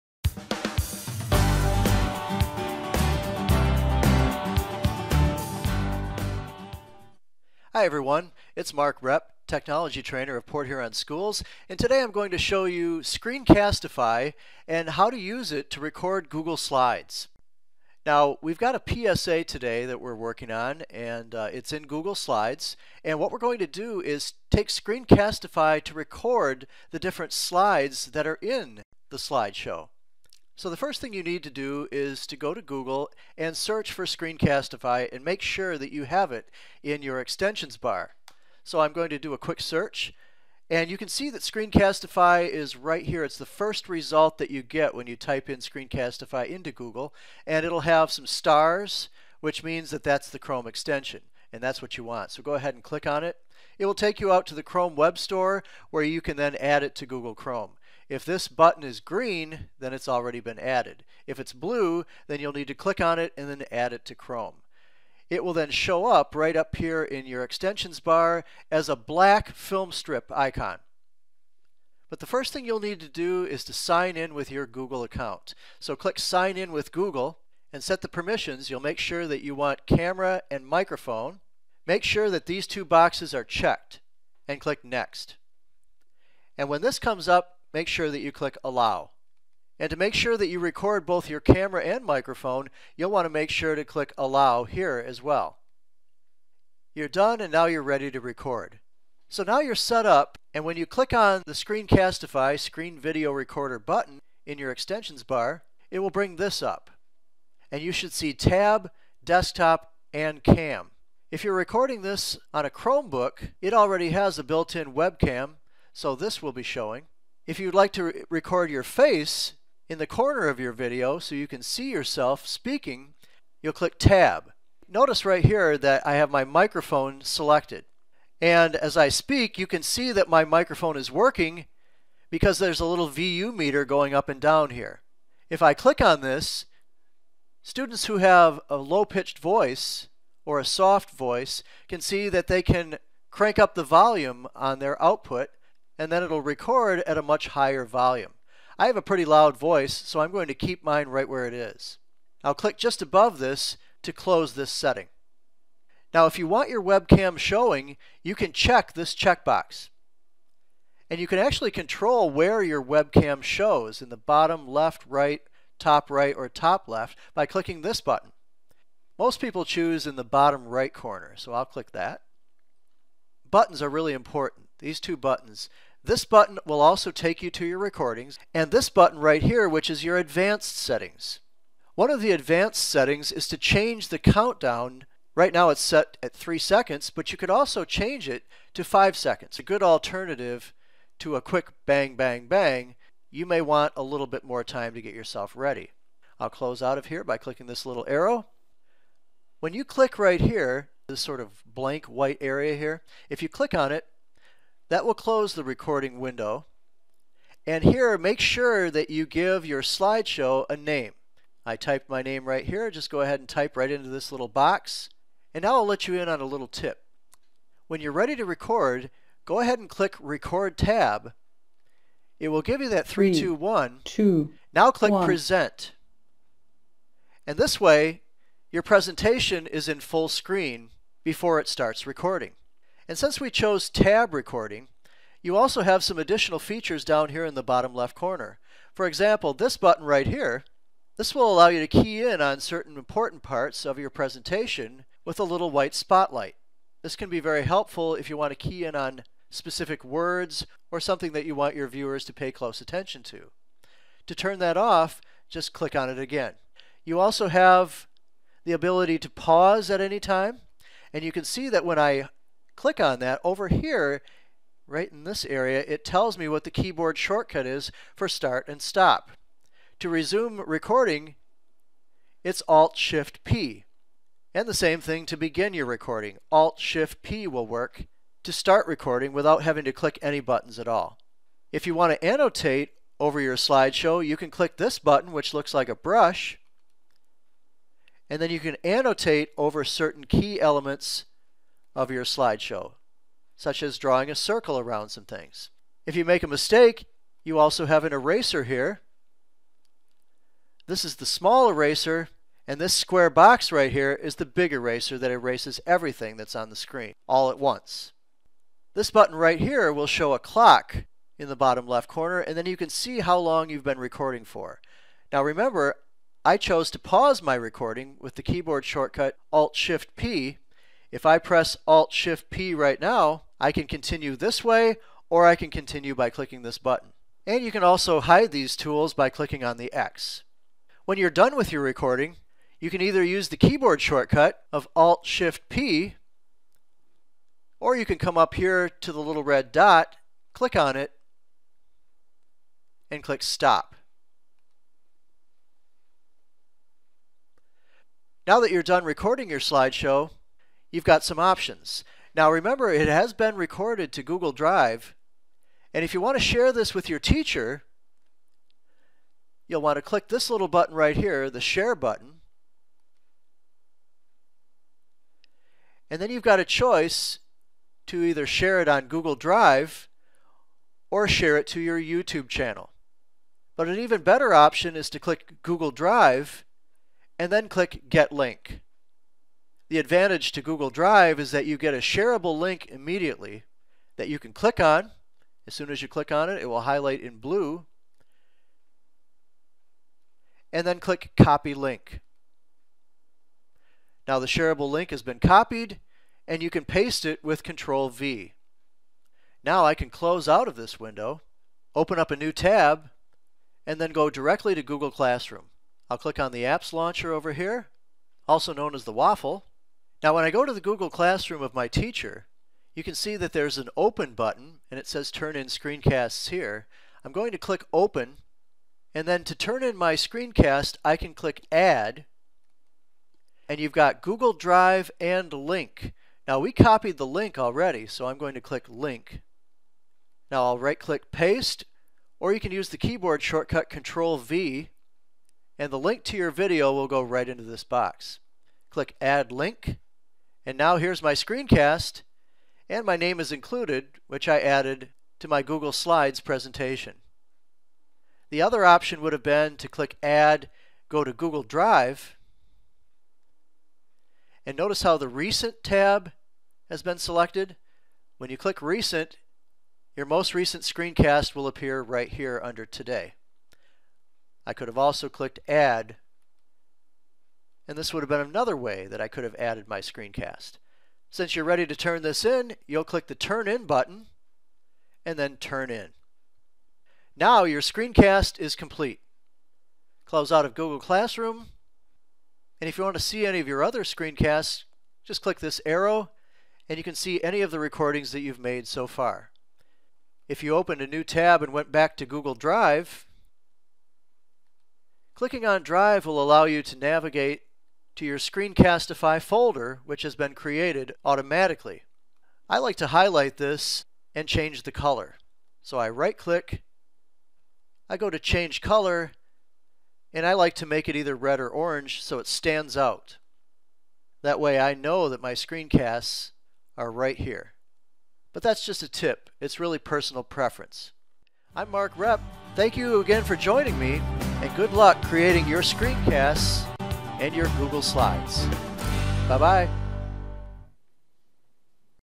Using Screencastify with Google Slides and Google Classroom 2018
This video tutorial shows the complete student workflow of recording a Google Slides Presentation using Screencastify and then turning in the video through Google Classroom two different ways. Many of the newest features of Screencastify's Tab recording toolbar are shown, including the timer, spotlight, pen, pause button, start and stop keyboard shortcut and the eraser.